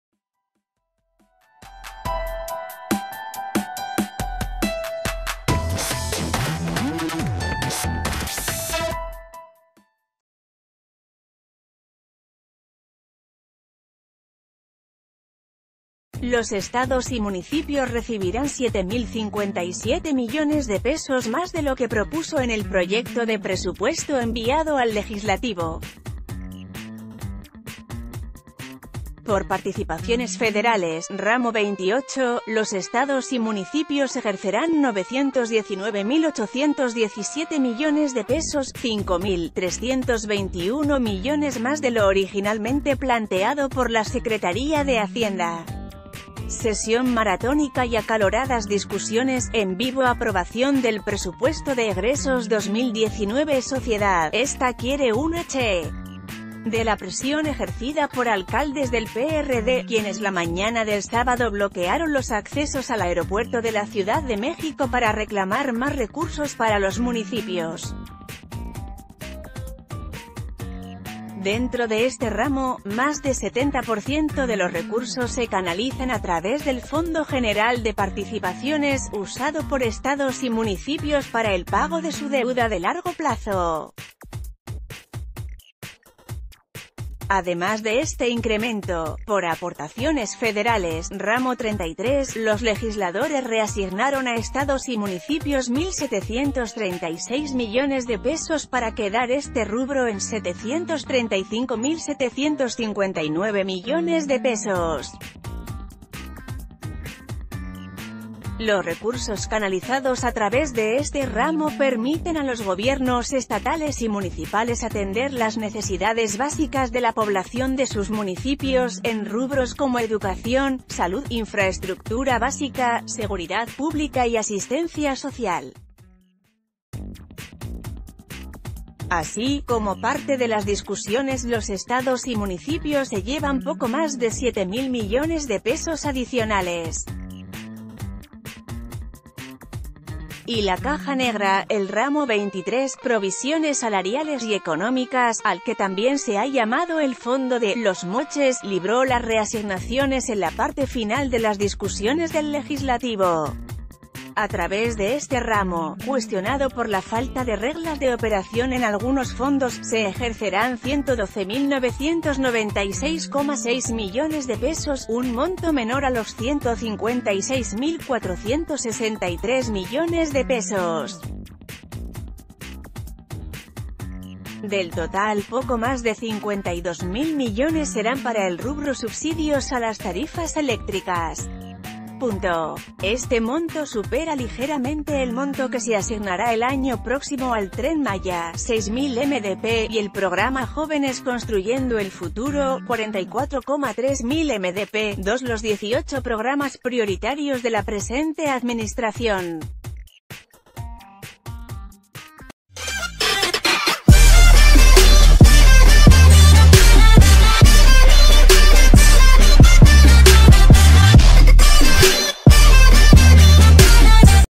Diputados asignan 7 mil mdp más a estados y municipios
Los estados y municipios recibirán siete mil 57 millones de pesos más de lo que propuso en el proyecto de presupuesto enviado al legislativo. Por Participaciones Federales (Ramo 28), los estados y municipios ejercerán 919 mil 817 millones de pesos, cinco mil 321 millones más de lo originalmente plan...